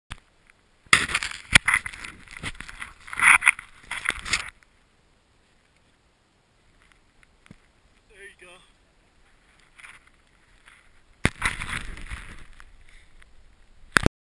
Russell B. 180
TBaggs497 pulling a smoove 180...and of course you can't see it. Not bad for an old timer.